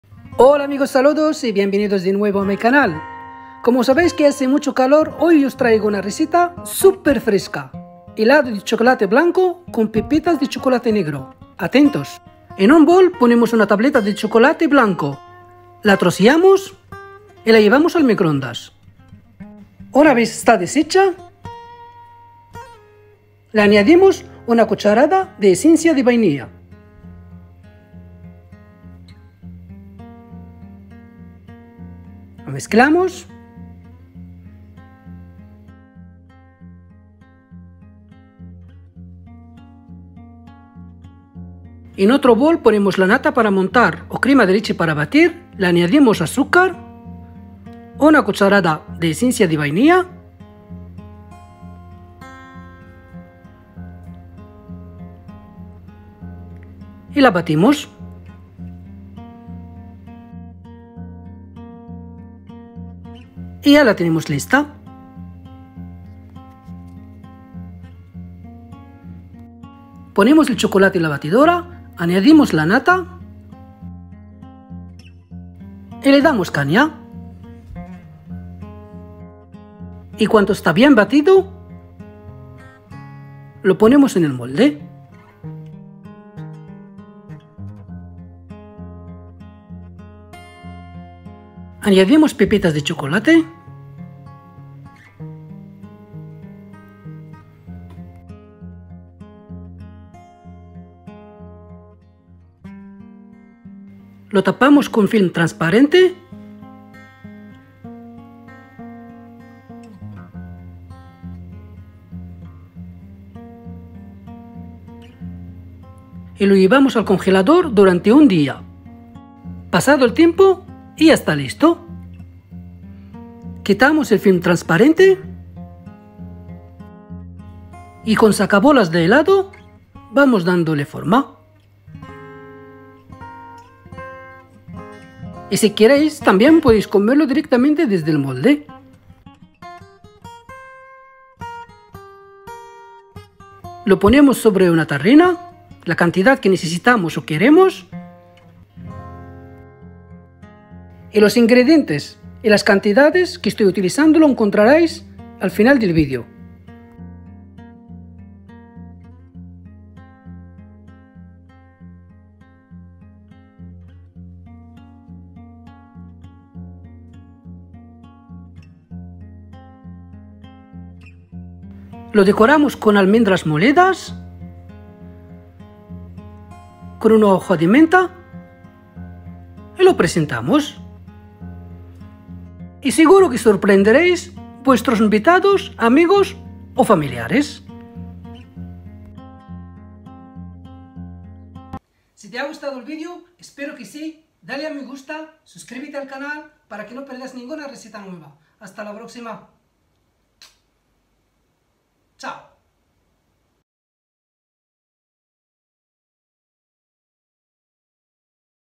HELADO DE CHOCOLATE BLANCO
Hola amigos, hoy os traigo una receta muy fresca helado de chocolate blanco con pepitas de chocolate negro, es una delicia

INGREDIENTES
500 ml. de nata para montar o crema de leche para batir
200 gr. de chocolate blanco
2 cda. esencia de vainilla
50 gr. de azúcar
Pepitas de chocolate
almendras molidas